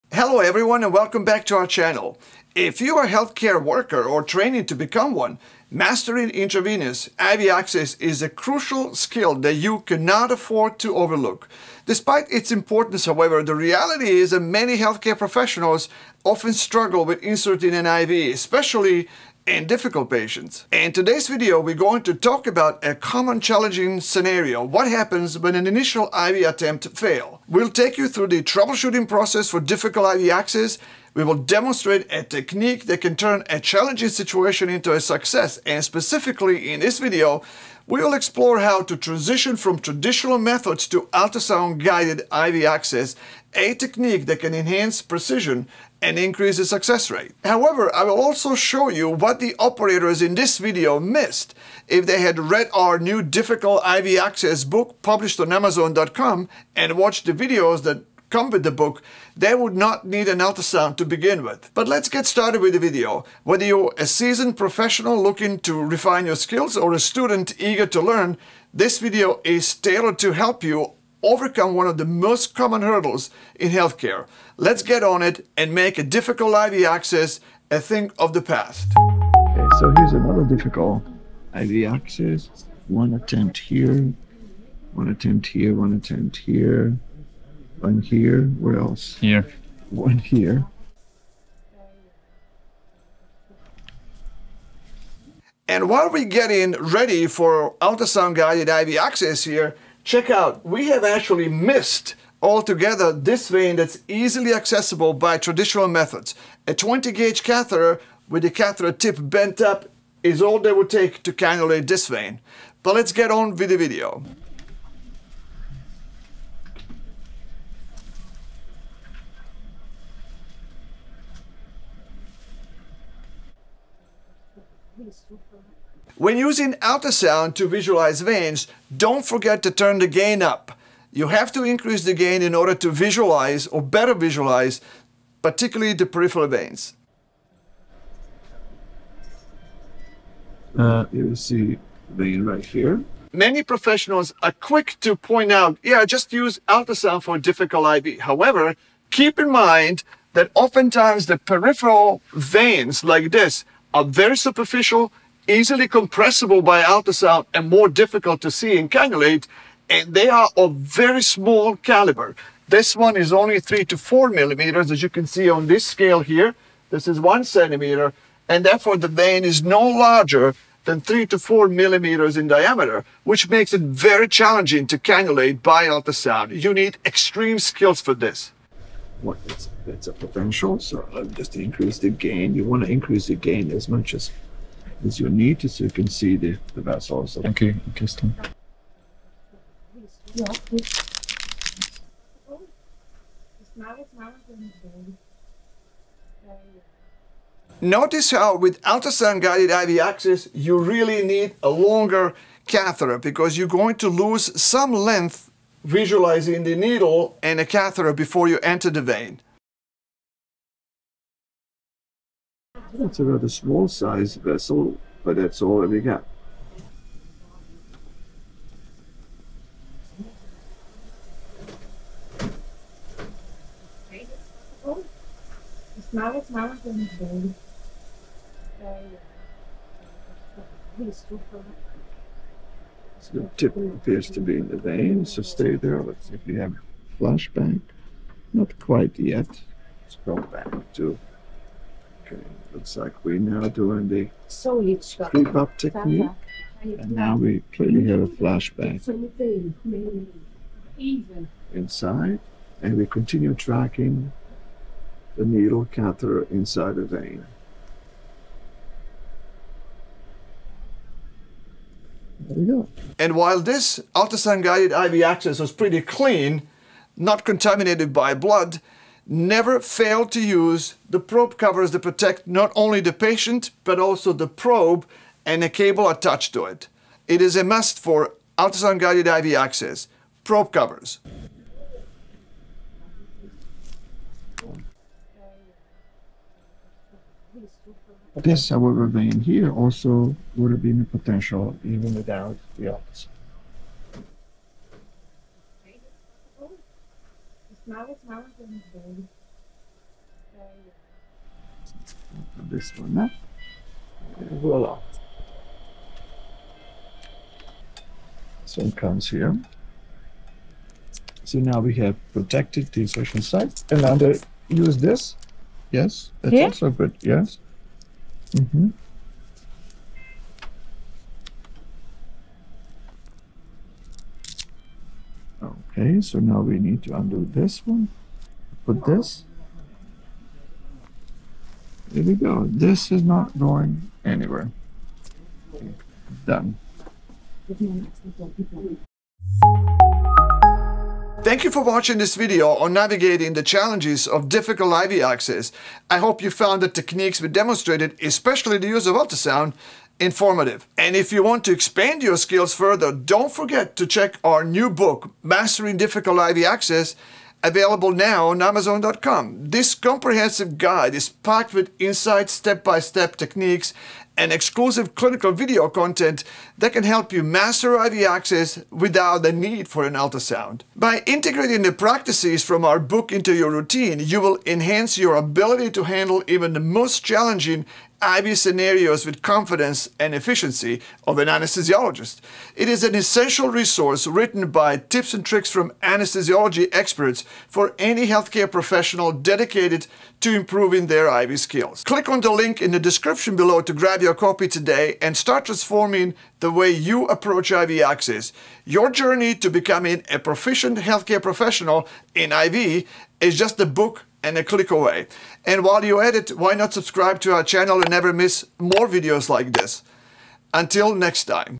DIFFICULT IV: TIPS FOR US-GUIDED IV ACCESS
In this video, we demonstrate the technique of peripheral access using ultrasound guidance. The video demonstrates the extreme skills of one of NYSORA’s regional anesthesia fellows, Dr Leander Mancel, who used a “creep up” technique to advance the catheter into a small peripheral vein with a caliber of only 3-4 mm. While the ultrasound technology in this case facilitated vein access, we also remarked that traditional methods, like simply applying a tourniquet and waiting for veins to become more prominent, could have been equally effective in this patient, obviating the need for ultrasound altogether. This highlights an essential point: traditional, non-ultrasound IV access remains indispensable and can be even more time-efficient in many clinical settings.